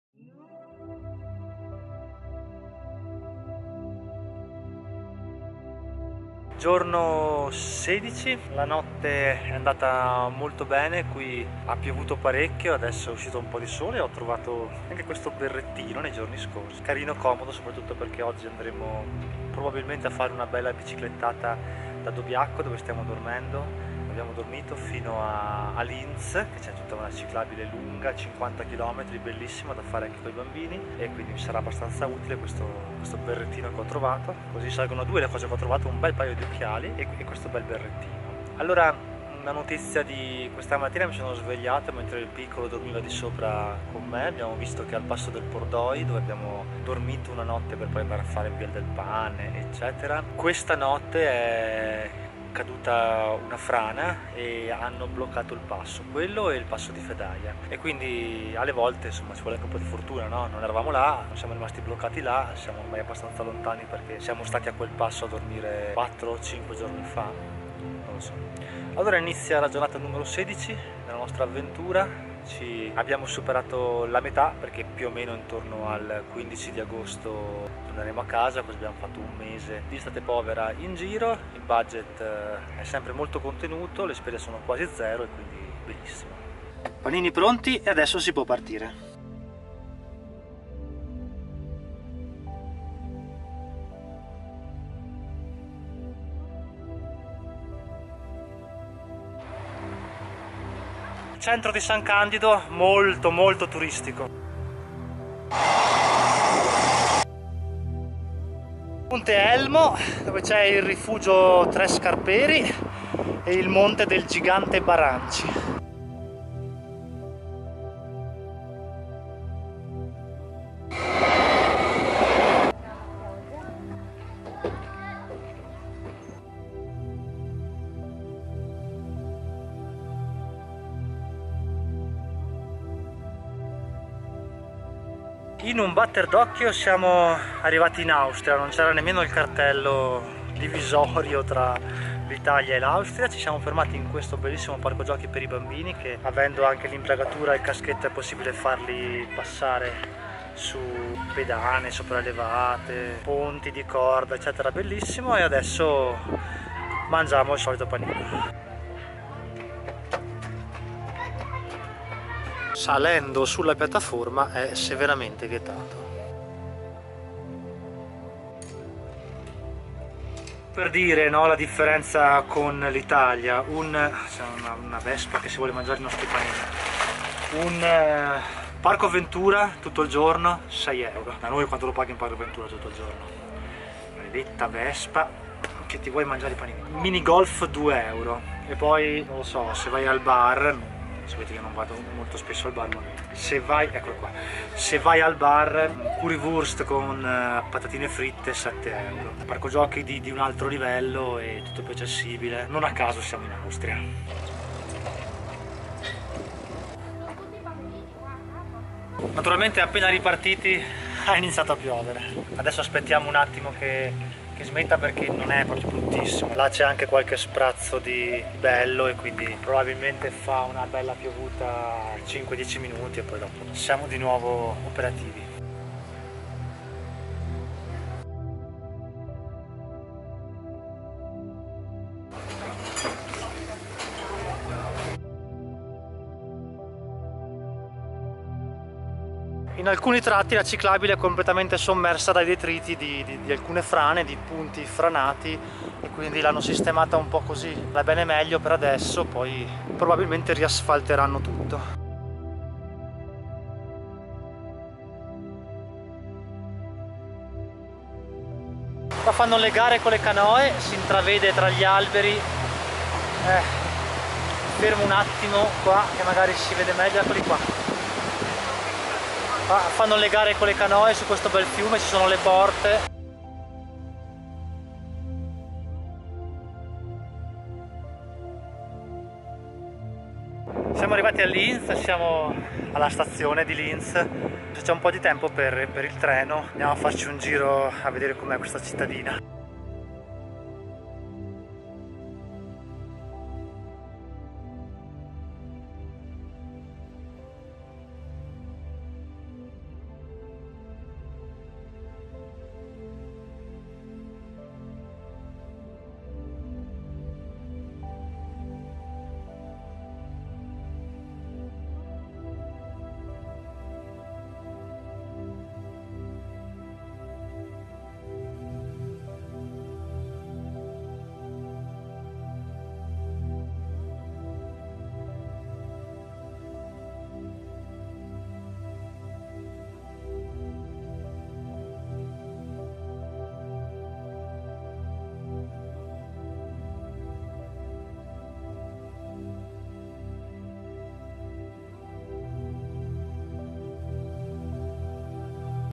Però... solo 31 euro al chilo? (giorno #16)
L'estate povera è il modo di viaggiare sostanzialmente gratis che abbiamo architettato per vivere nella semplicità volontaria e non rinunciare alle emozioni del viaggio. In questa serie di 30 video vi racconto la mia estate povera 2020 sulle Dolomiti.

ATTENZIONE: I commenti irrispettosi, ignoranti, palesemente volti a ricercare lo scontro o che non danno un reale contributo alla community vengono eliminati.